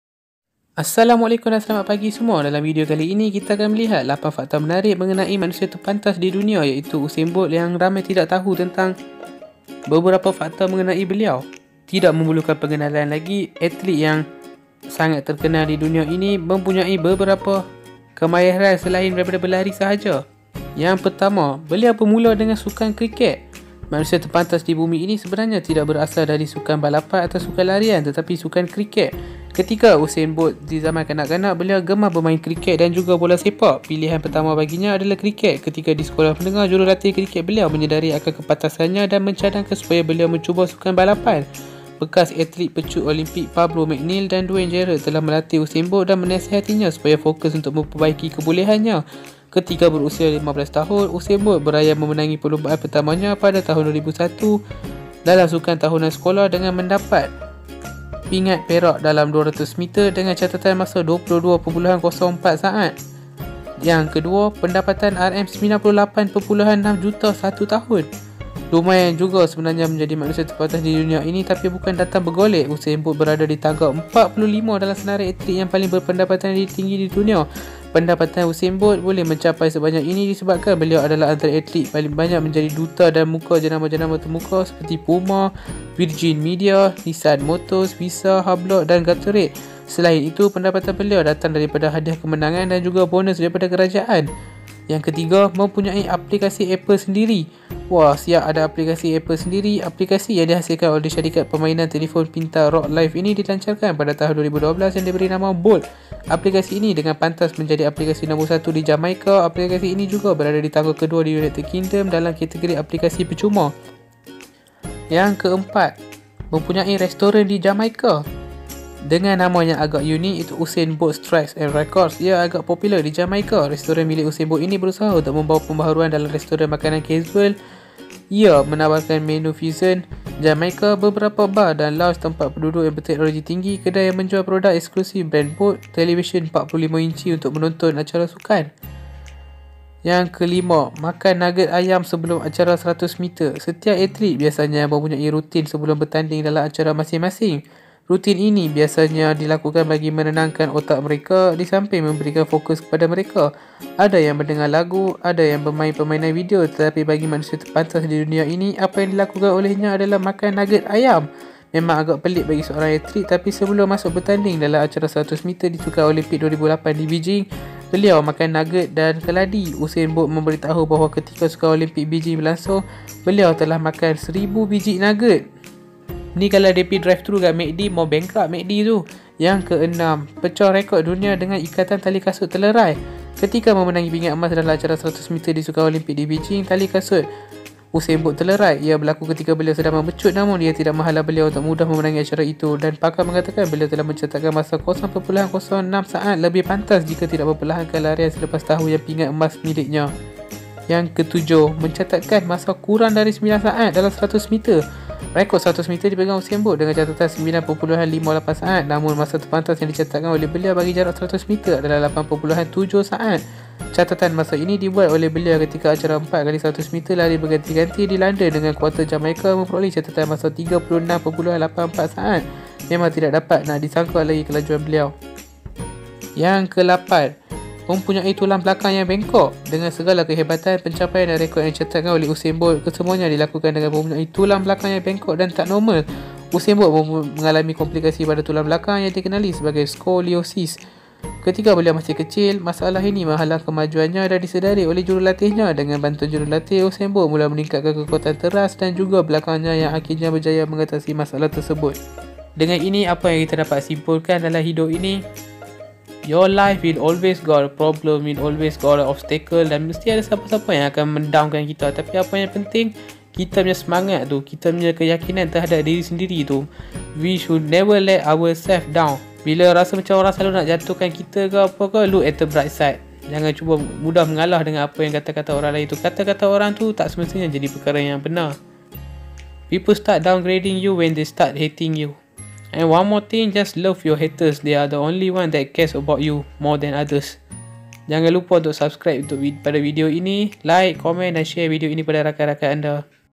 8 Fakta Menarik Usain Bolt | Manusia Terlaju Di Dunia | Bahasa Melayu | Malaysia | Fakta Faheem #53
Hai teman teman dalam video kali ini kita akan membincangkan tentang beberapa fakta menarik yang tidak diketahui ramai tentang seorang atlet atau lebih dikenali sebagai manusia terlaju di dunia iaitu Usain Bolt.

Juga motivasi untuk berjaya kepada korang yang tengok video ini sampai habis.

Apa pun, kesemua ni pendapat aku yang tersendiri la. Masing- masing ada pendapat yang berbeza. So dia depends dengan korang sendirila.

Semoga Video kali ini bermanfaat buat teman teman sekalian, Assalammualikum.